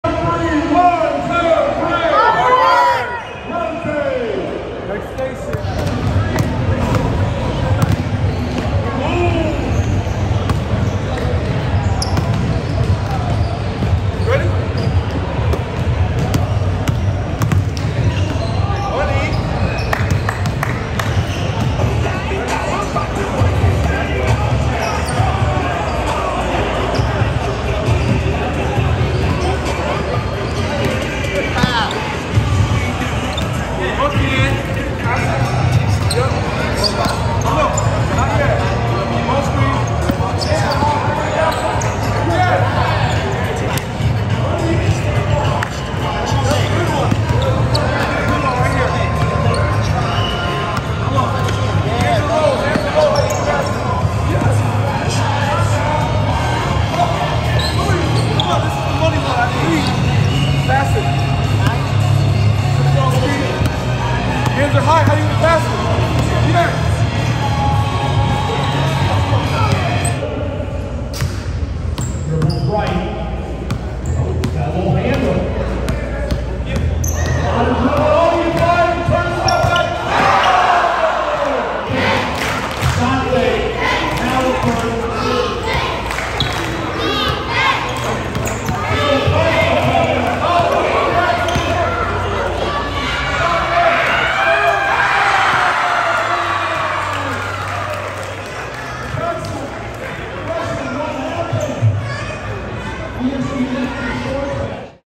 Tyrese Haliburton Youth Basketball Camp Highlights 2022
Highlights from Tyrese Haliburton's first basketball camp in Indy — held on August 13, 2022 at the Pacers Athletic Center in Westfield, Ind.

Scott Agness runs FieldhouseFiles.com covering the Pacers.